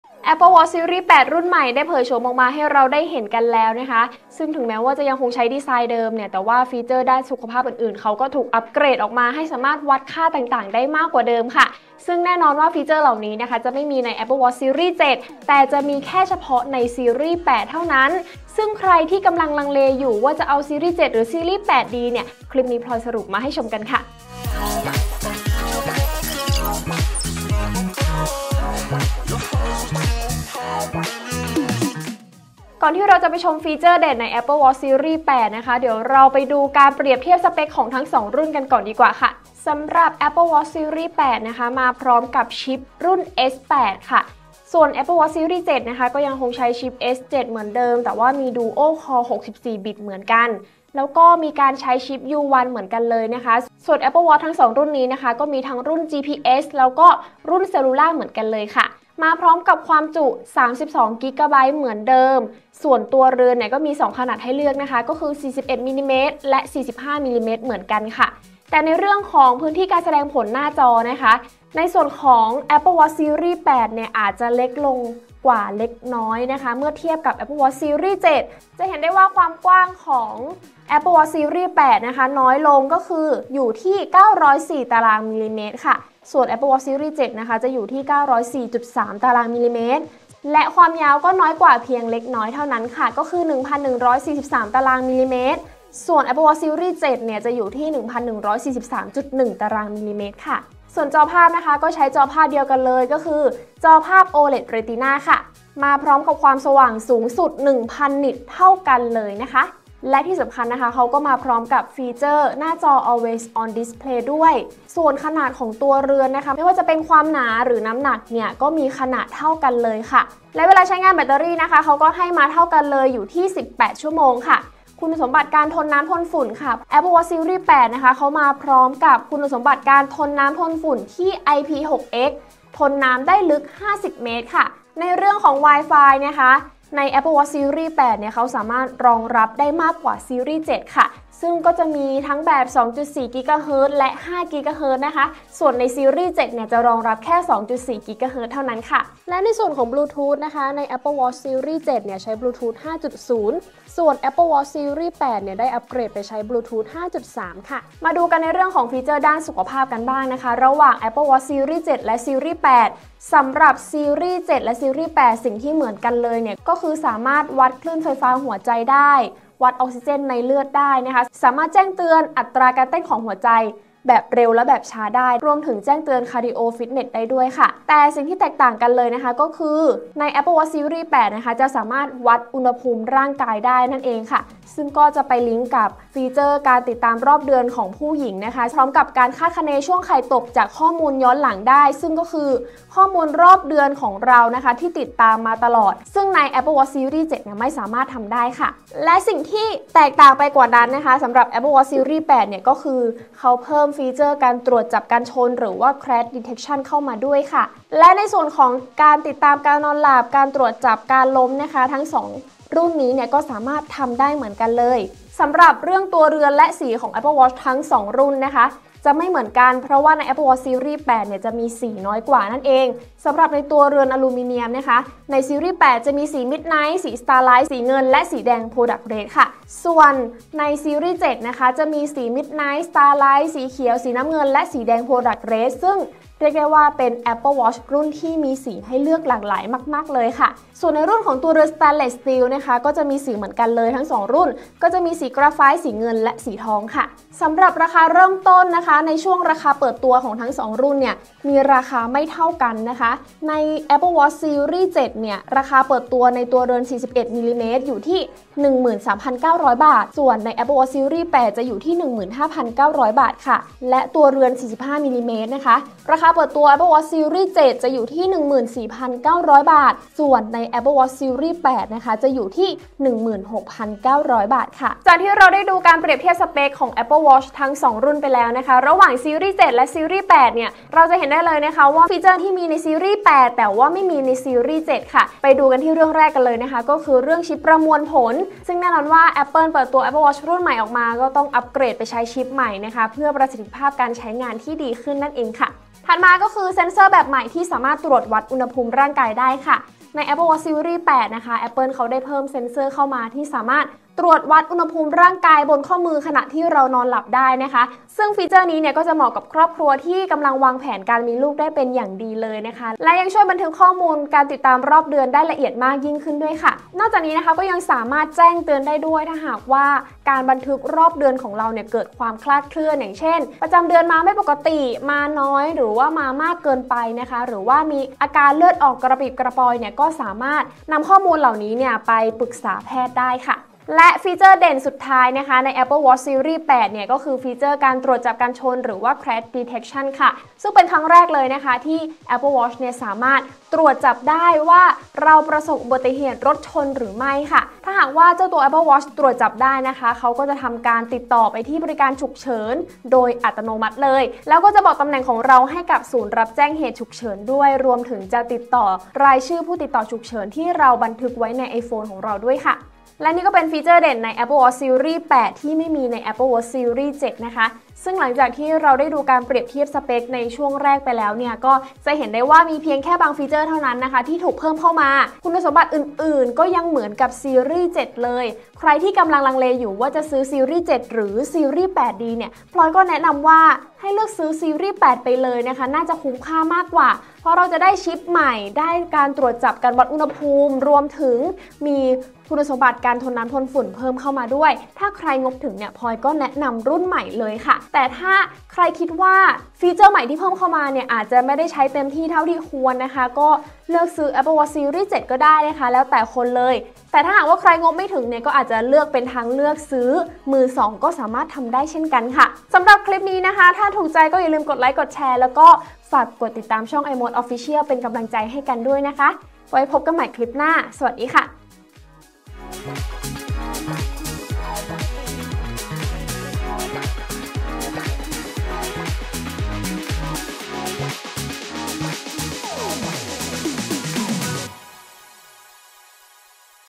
ชมฟีเจอร์เด็ด! ใน Apple Watch Series 8 ที่ Series 7 ไม่มี! | iMoD
Apple Watch Series 8 เปิดตัวแล้ว! แม้จะใช้ดีไซน์เดิม แต่ก็มีการอัปเกรดฟีเจอร์ด้านสุขภาพมาใหม่ ซึ่งฟีเจอร์เหล่านี้ไม่มีใน Apple Watch Series 7 มีแค่ใน Series 8 รุ่นใหม่เท่านั้น! มีอะไรบ้าง ไปชมกัน

00:00 - เปิดคลิป
00:32 - เปรียบเทียบ Apple Watch Series 8 vs. Series 7
04:07 - ราคาเปิดตัว
04:48 - ชิปประมวลผล
04:55 - เซ็นเซอร์ตรวจวัดอุณหภูมิร่างกาย
05:33 - ฟีเจอร์ตรวจจับการชนกัน